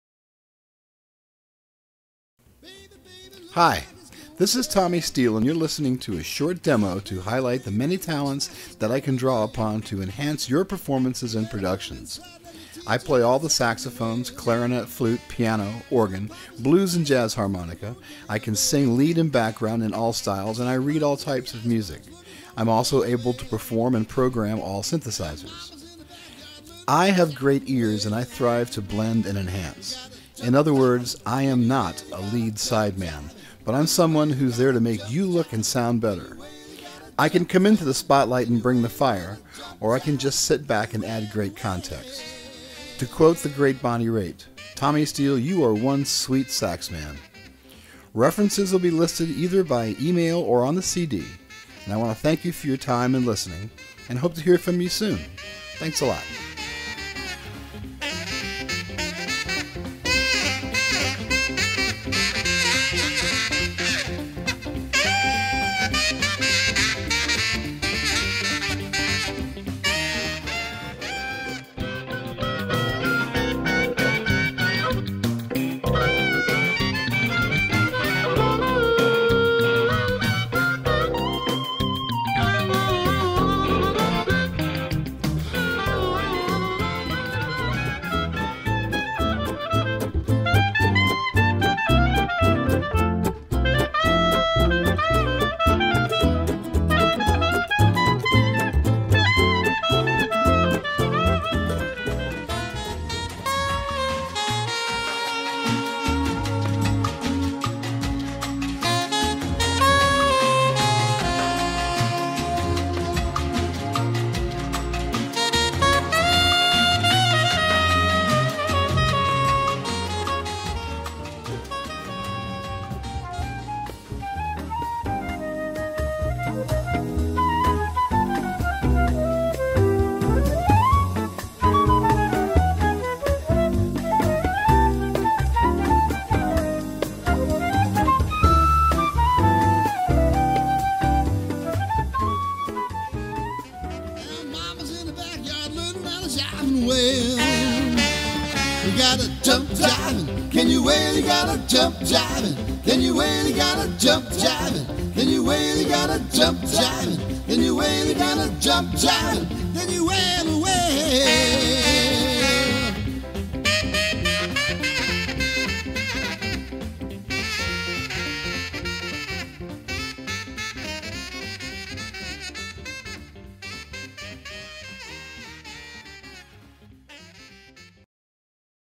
Tommy Steele Musician
Las Vegas based multi-talented.  Currently looking to pick up sub-work or a new show gig.  Works with Kelly Clinton, Las Vegas Tenors, Cliint Holmes, Shades of Sinatra  - but always trying to find more peopleto work with.